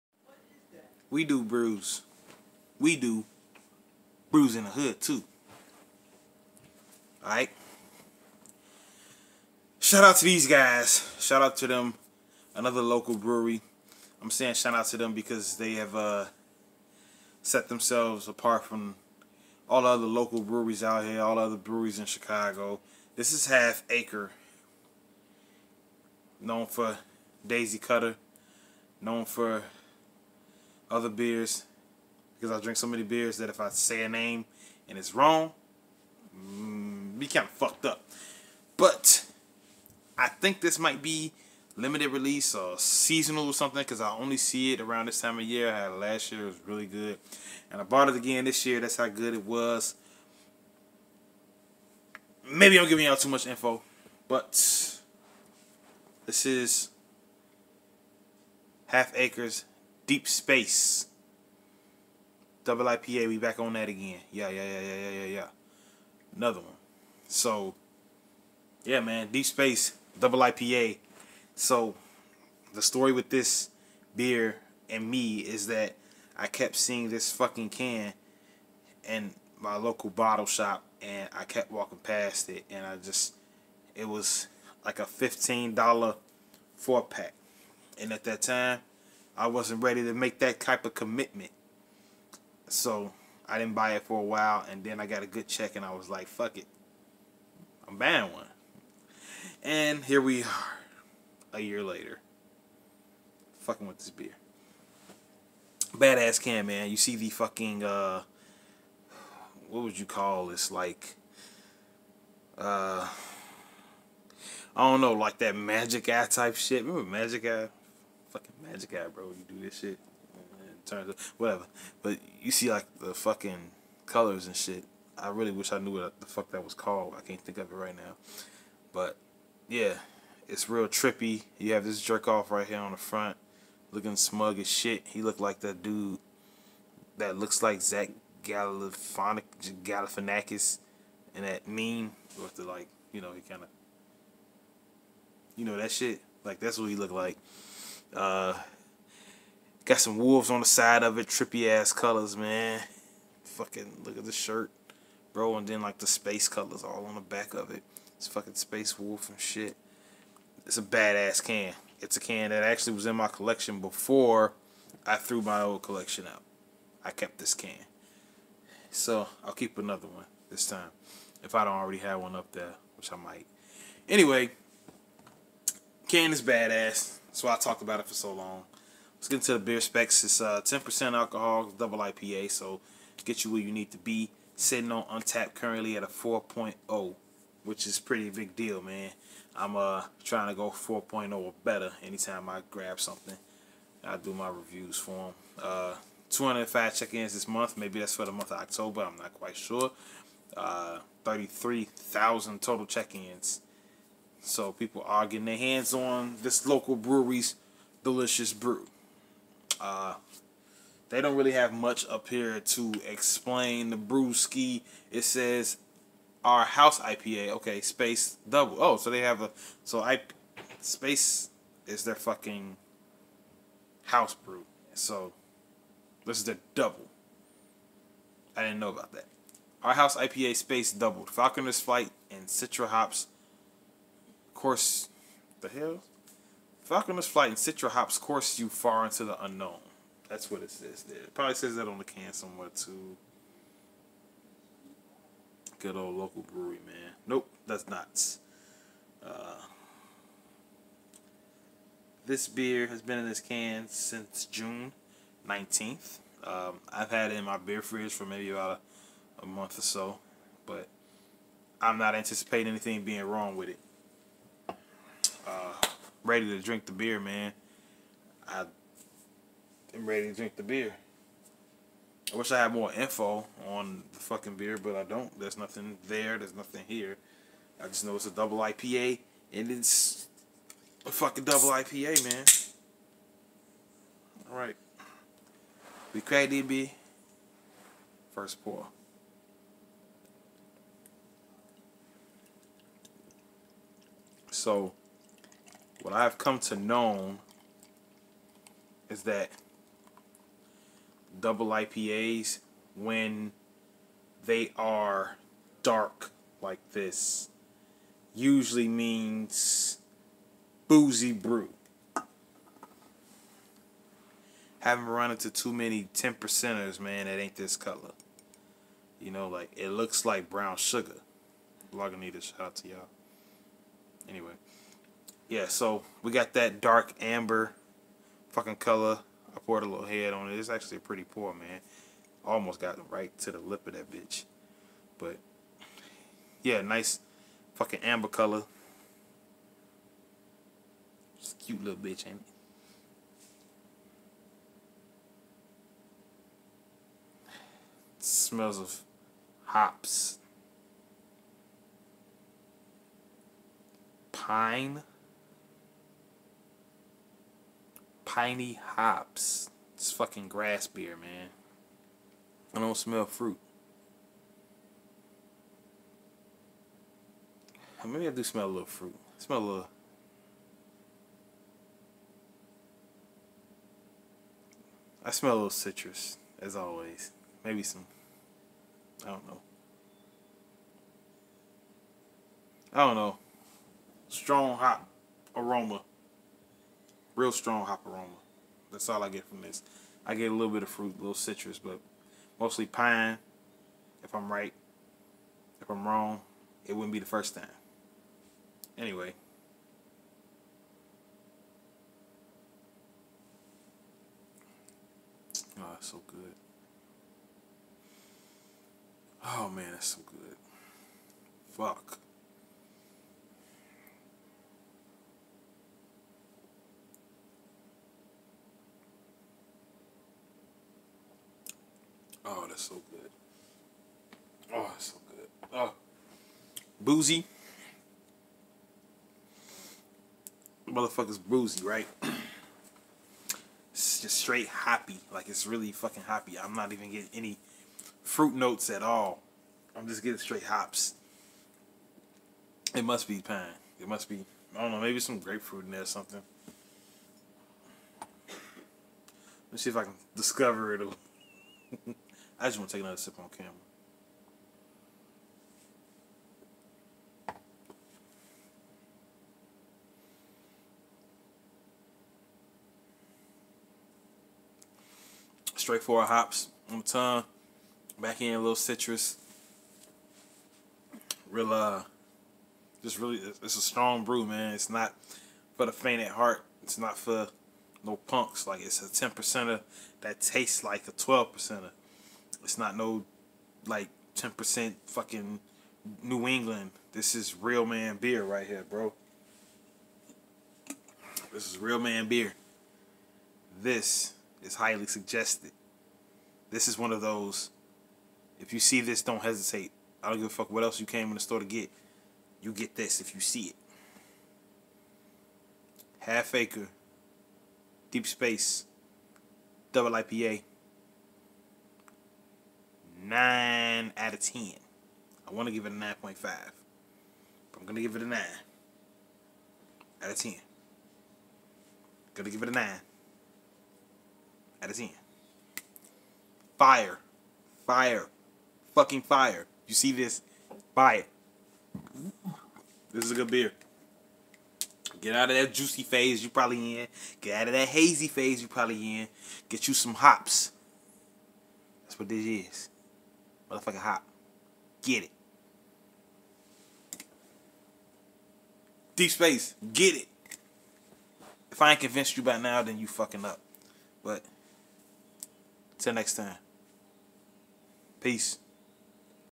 Brews in the Hood - Deep Space Double IPA (Half Acre Brewing Co)
Half Acre Brewing Co out of Chicago, Il.

Notes: Imperial IPA, a doubling of our house IPA, Space. A chasm of pits and peaks, swollen with caramel and sap. The water is chummed, here come the sharks